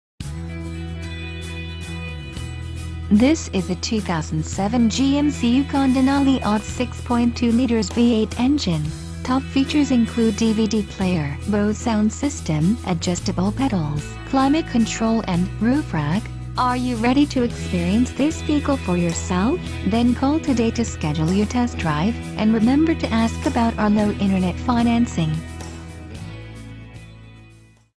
2007 GMC Yukon in Gladstone OR
At Auto Town Buick GMC , our long-standing customers know us a dependable source of used pre owned vehicles, like this GMC Yukon Denali AWD4 Door , conveniently located in Gladstone, Oregon . For the absolute best in service, selection, and value, please visit our to learn more about this exceptional 2007 GMC Yukon

2007 GMC 4 Door Denali AWD4 Door

Engine: V8, 6.2L
Fuel: Gasoline
Transmission: Automatic
Exterior Color: White
 Stock #: GP3660
 VIN: 1GKFK63877J254659